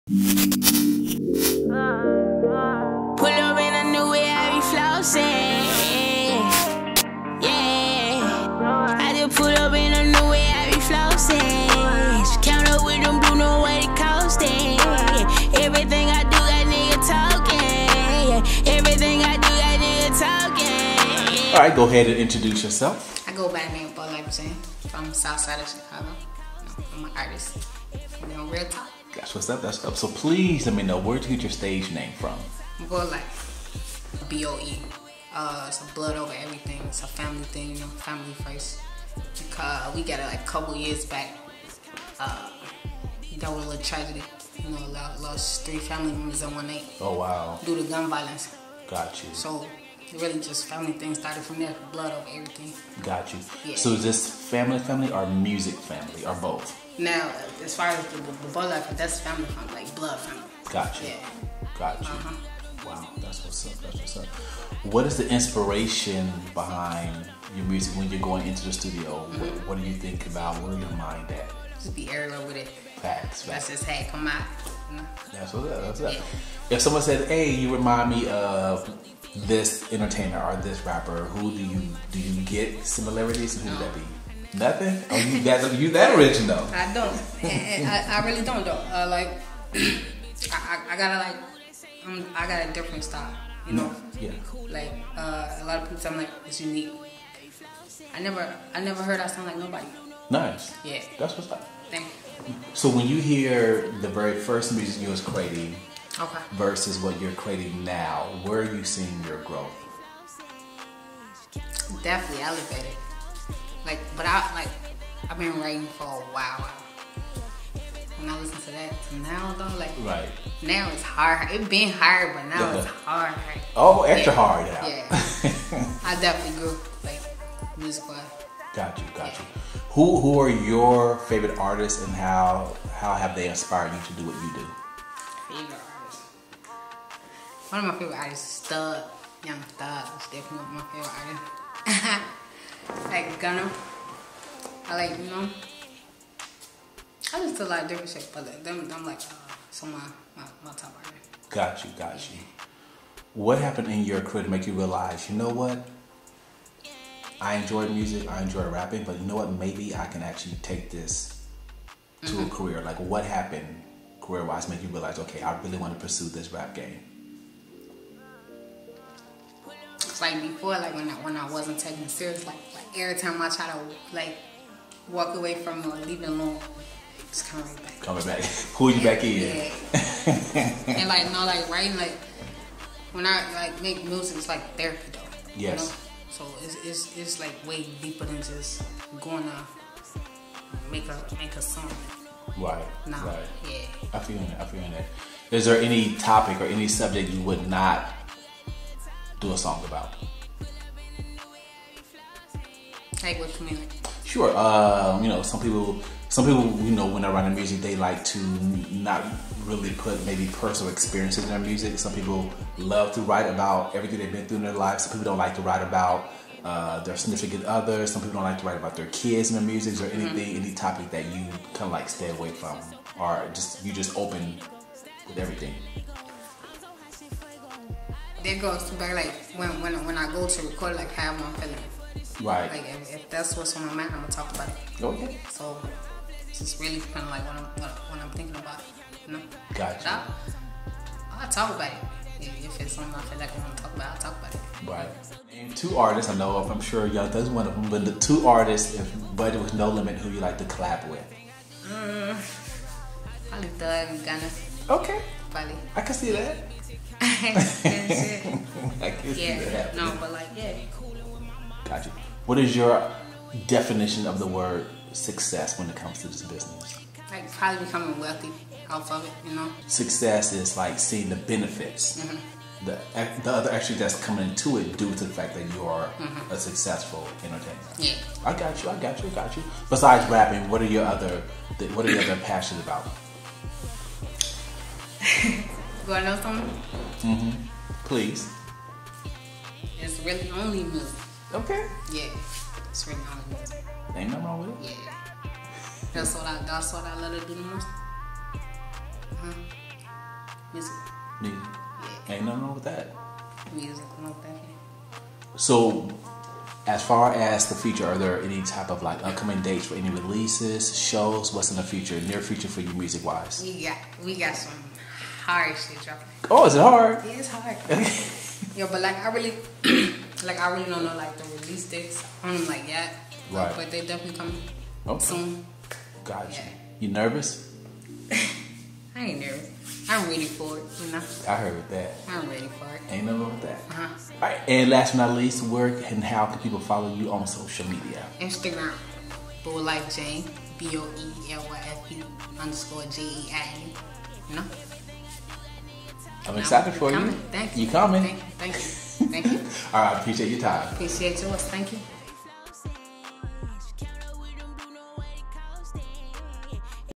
Reel Talk Presents: BoeLyfe
Interviewed by: Quar "QB" Brown @quarbrown
Interview session of @boelyfe_jaine
Discussing musical her style, inspiration and the future